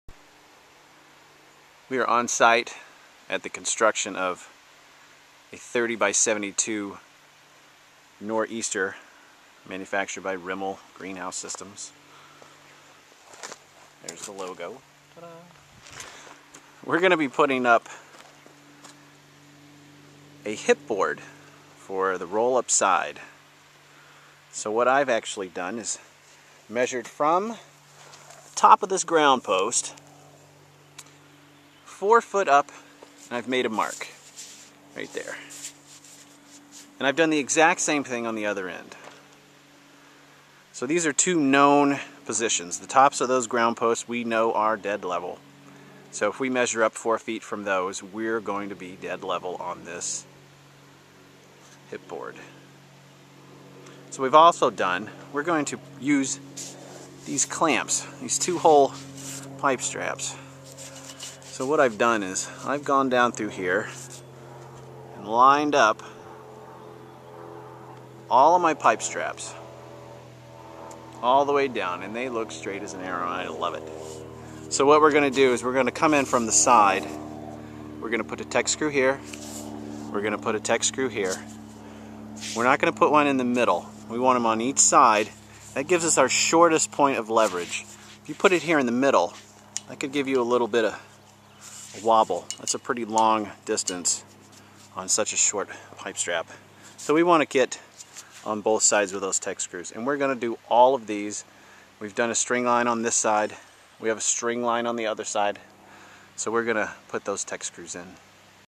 Greenhouse Construction: Rimol Nor'Easter Construction Pt. 5
Our greenhouse construction series takes a look at the process of installing roll-up sides.